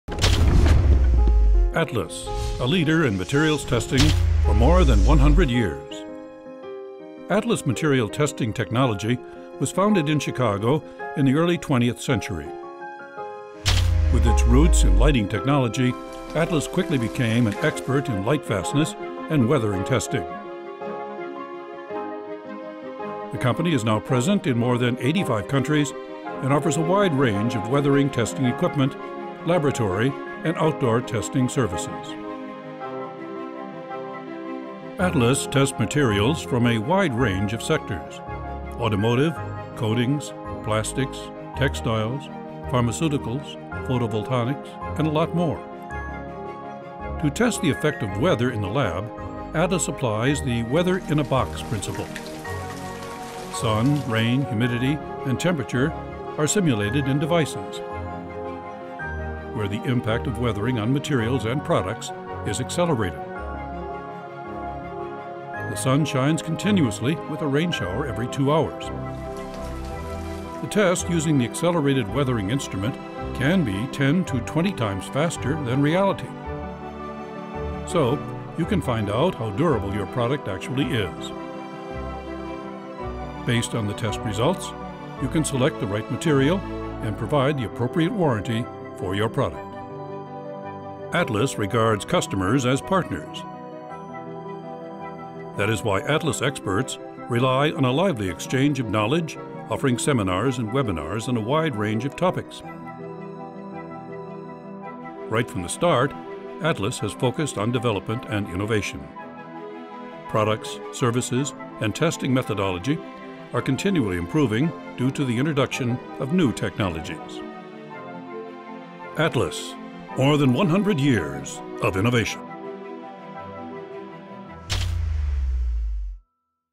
Atlas Material Testing Technology | Weathering Testing Solutions | Overview [English, Long Version]
For more than 100 years, Atlas has been a leader in materials durability testing, offering a complete line of weathering testing instrumentation, laboratory, and outdoor testing services. Atlas products are designed and manufactured to conform to international and industry standard test methods, including ISO, ASTM, DIN, JIS and numerous others. Markets include automotive, paint and coatings, plastics and additives, textiles, pharmaceutical and consumer products, architecture, aerospace, photovoltaics, and packaging.